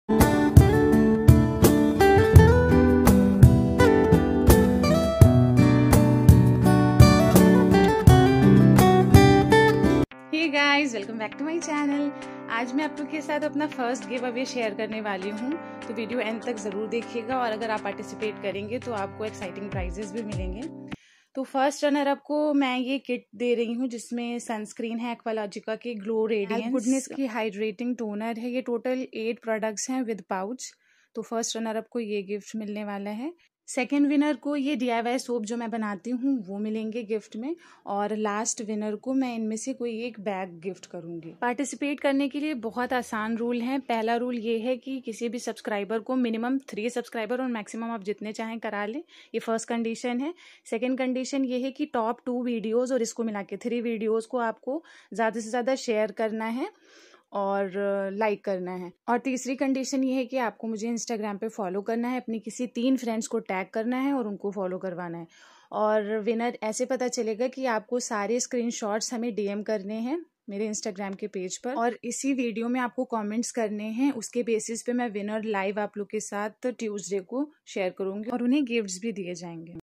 My First Giveaway on YouTube || Join now & Win Prizes
S tuber
1st condition - make at least 3 subscribers and send the screenshot at my Instagram account.
You all have to send me the screenshot of all three conditions and  MOST IMMORTALITY
Winners will be announced live on Tuesday.

Prizes 🎁🎁🎁
1st Winner -
1.Aqualogica Sunscreen
2.Alps goodness hydrating toner
3.Earrings
4.Gel eyeliner
5.Good vibes Gold Peeloff mask
6.Six eye & lip liners
7. 1 Face Razor
8. Mcaffeine facewash / scrub
9. 1 carry pouch 👝
2nd Winner - DIY soaps 🧼
3rd Winner - Bag 👜
Note- Everything you see in this video is created by me i.e s tuber. So kindly do not copy any picture or video without my permission. 🙏